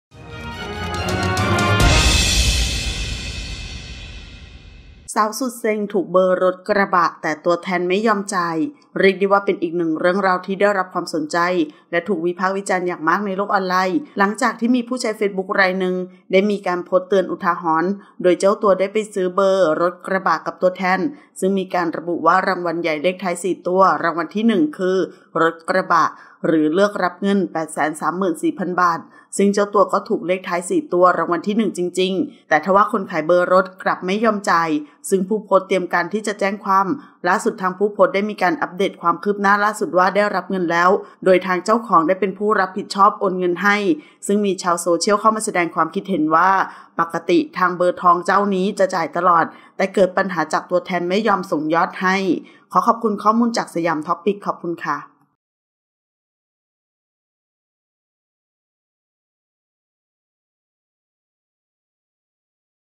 สาวสุดเซ็ง ถูกเบอร์รถกระบะ แต่ตัวแทนไม่ยอมจ่าย
เรียกได้ว่าเป็นอีกหนึ่งเรื่องราว ที่ได้รับความสนใจ และถูกวิพากษ์วิจารณ์อย่างมากในโลกออนไลน์ หลังจากที่มีผู้ใช้เฟซบุ๊กรายหนึ่ง ได้มีการโพสต์เตือนอุทาหรณ์ โดยเจ้าตัวได้ไปซื้อเบอร์รถกระบะกับตัวแทน ซึ่งมีการระบุว่ารางวัลใหญ่เลขท้าย 4 ตัวรางวัลที่ 1 คือ รถกระบะ หรือเลือกรับเงิน 834,000 บาท ซึ่งเจ้าตัวก็ได้ถูกเลขท้าย 4 ตัวรางวัลที่ 1 จริงๆ แต่ทว่าคนขายเบอร์รถกลับไม่ยอมจ่าย ซึ่งผู้โพสต์เตรียมการที่จะแจ้งความ

ล่าสุดทางด้านผู้โพสต์ได้มีการอัปเดตความคืบหน้าล่าสุดว่า ได้รับเงินแล้ว โดยทางเจ้าของได้เป็นผู้รับผิดชอบโอนเงินให้ ซึ่งก็มีชาวโซเชียลเข้ามาแสดงความคิดเห็นว่า ปกติทางเบอร์ทองเจ้านี้จะจ่ายตลอด แต่เกิดปัญหาจากตัวแทนไม่ยอมส่งยอดให้

ขอขอบคุณข้อมูลจาก: siamtopic

00:00 - Intro
00:10 - News
01:00 - Minute